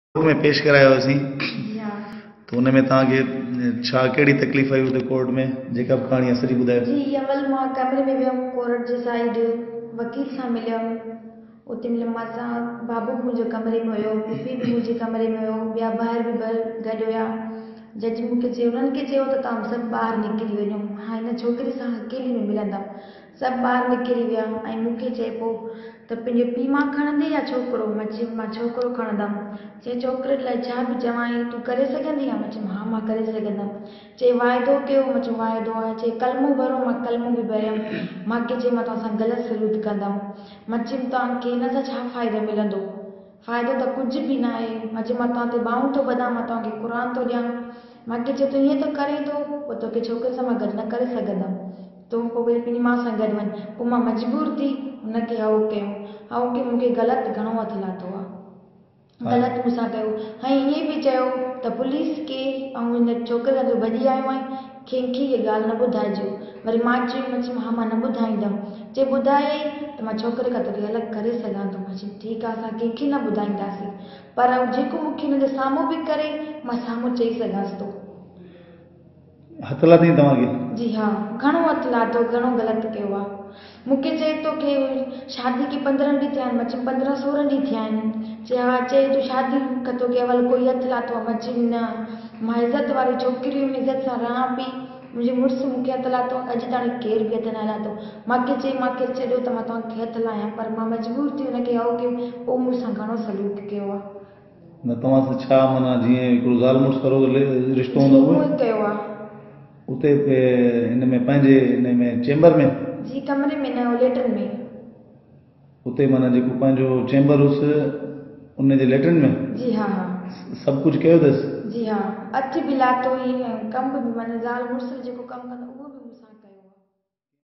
Salam Brohi video statment
Rape victim's statement , she is a village girls ,rapped by judge in his chamber . Instead of supporting her people started to accuse and abuse her , let's support her and demand